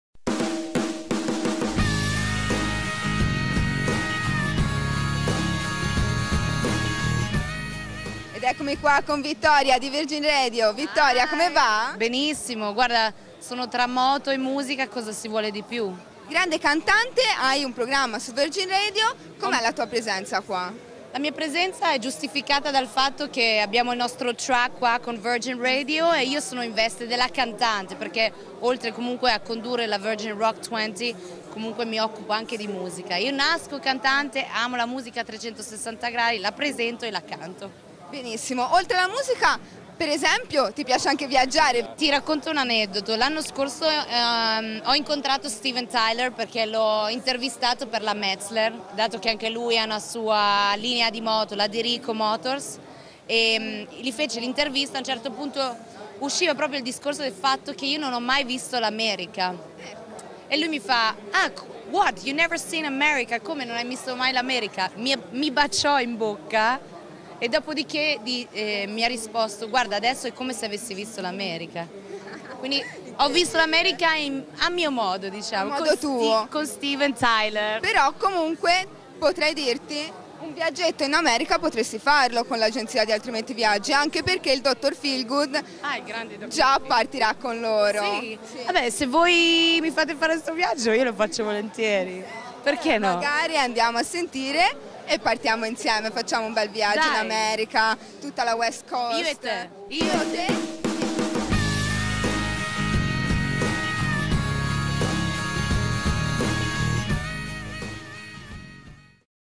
INTERVISTA Vittoria Hyde Virgin Radio EICMA 2014
Jessika Harleen per Garageschool Tv intervista Vittoria Hyde di Virgin Radio, ospite ad EICMA 2014

VISITATE LA NOSTRA PAGINA FACEBOOK!!!  :D